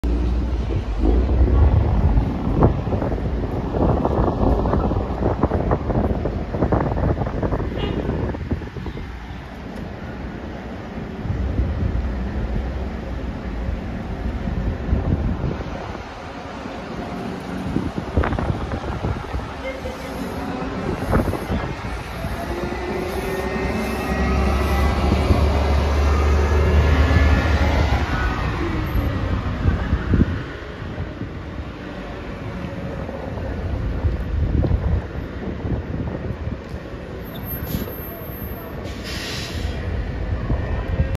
RTC LAS VEGAS TRANSIT: 2017 New Flyer XN60 on Route 117 to Las Vegas Strip
RTC LAS VEGAS TRANSIT: 2017 New Flyer XN60 on Route 117 to Las Vegas Strip express lasvegas shuttlebus newflyerxcelsior NewFlyerXN60 lasvegastransit route117 bus buses s